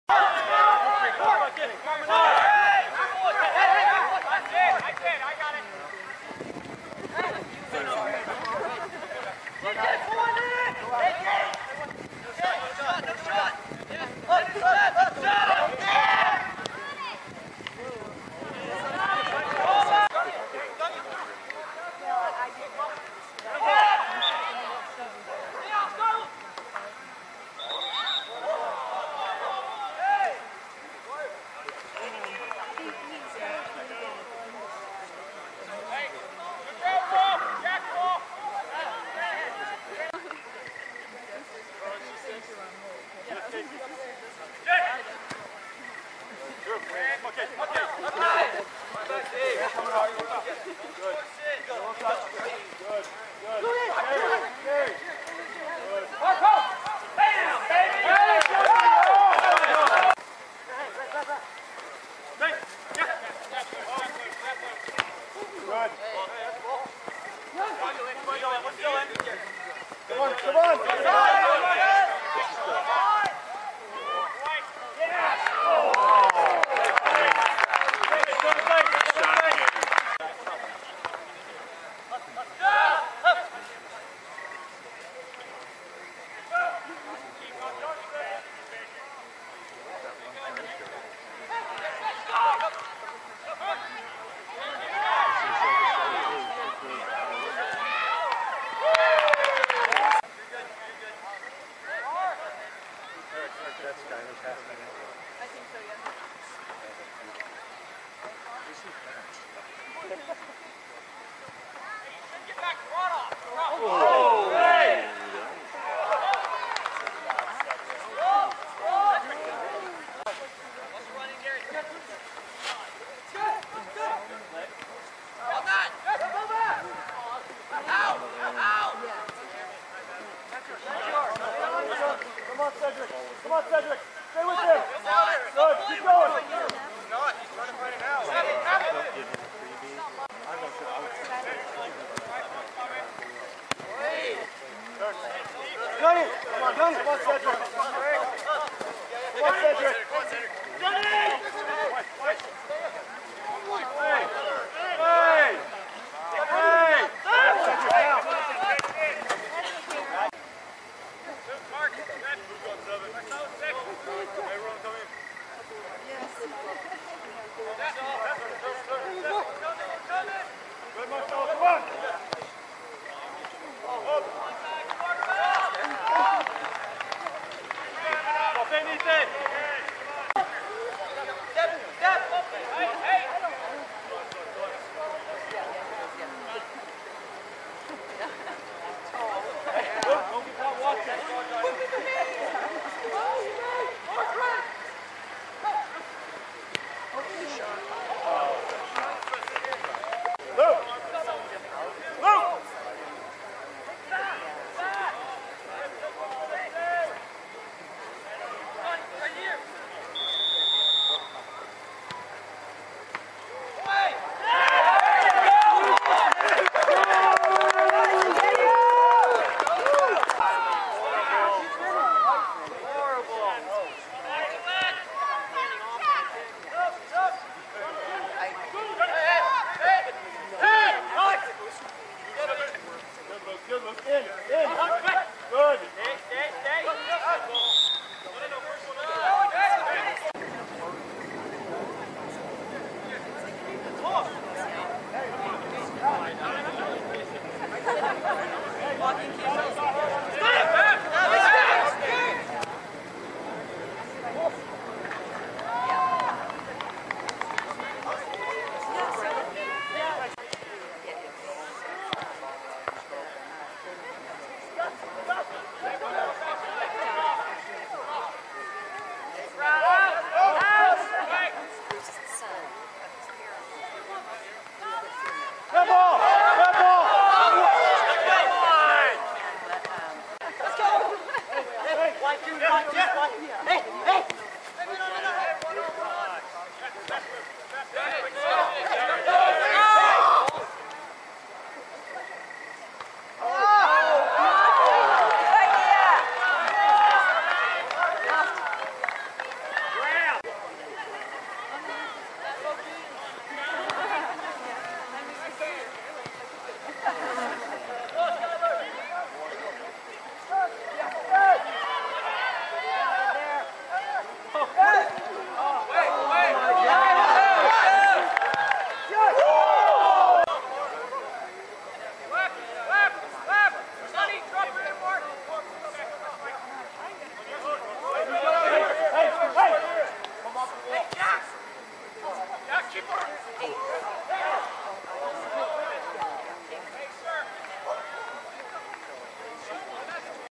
Timberline27Sep2016
Centennial Soccer Highlights Loss 0-1 to Timberline